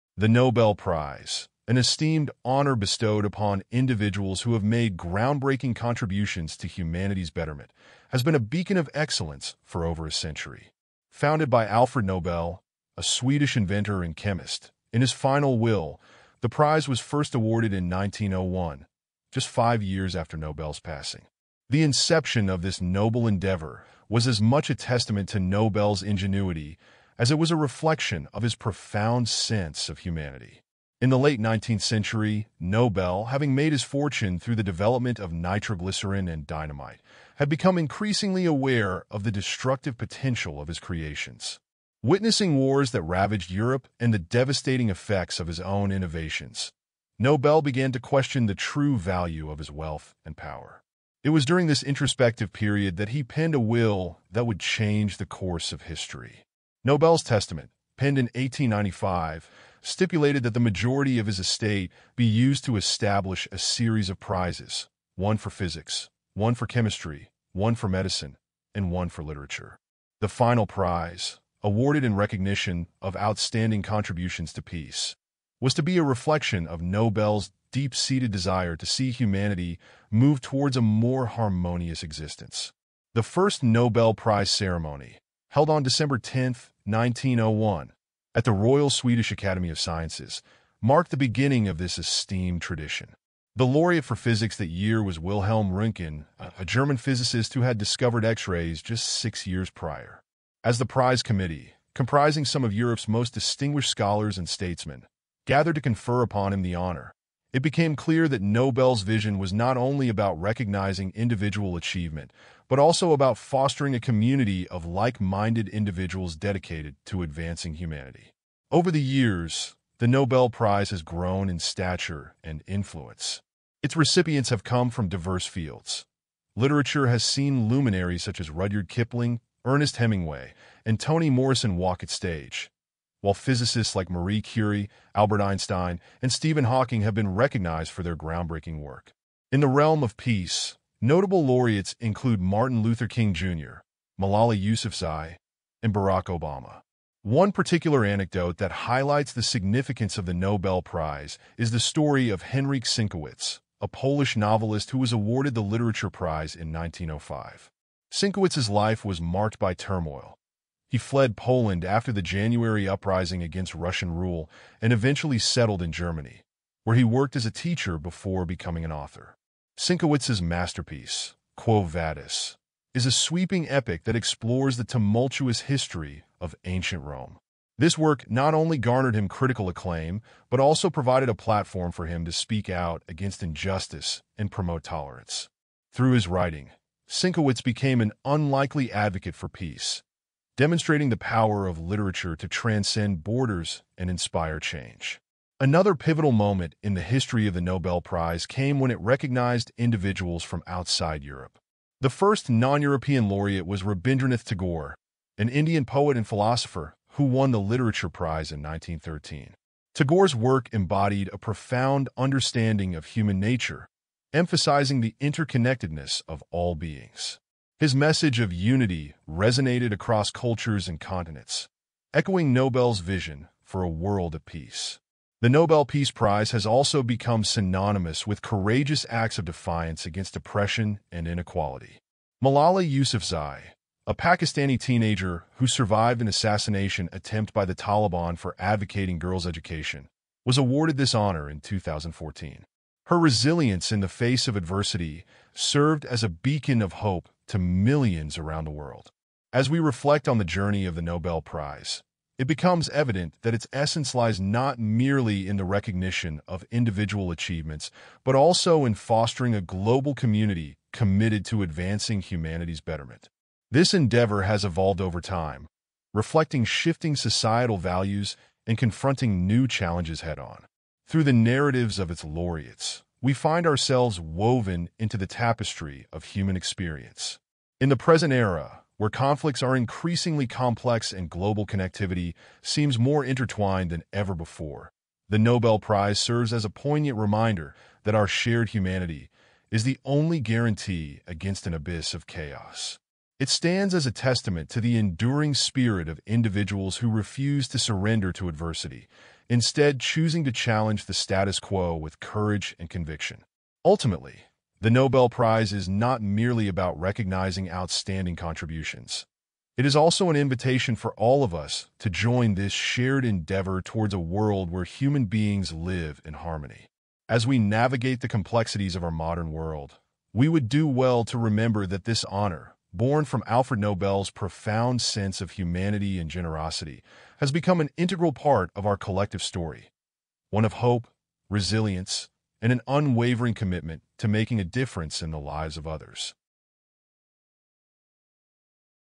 The Story Behind the Nobel Prize: A Legacy of Humanity
Discover how the Nobel Prize has been changing lives for over a century, recognizing groundbreaking contributions in physics, chemistry, medicine, literature, and peace. From Alfred Nobel's vision to the courageous acts of defiance against oppression, this esteemed honor has become a beacon of hope for humanity.

Join us on a journey through the stories of Nobel laureates like Marie Curie, Martin Luther King Jr., Malala Yousafzai, and Barack Obama, as we explore how their work has shaped our world. Learn about the significance of the Nobel Peace Prize in promoting unity and challenging injustice.

In this video, you'll also discover:

The humble beginnings of the Nobel Prize and its evolution over time The impact of Nobel laureates on society and their legacy The importance of recognizing outstanding contributions to humanity's betterment

What do you think is the most important aspect of the Nobel Prize? Share your thoughts, questions, or experiences related to this topic. We'd love to hear from you!